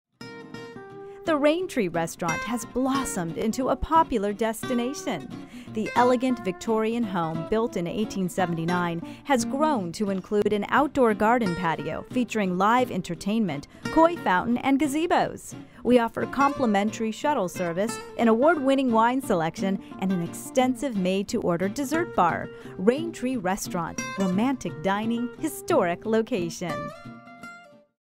St. Augustine Restaurants - The Raintree Restaurant
TV spot for the Restaurant with a Voice over and a focus on the experience and atmosphere.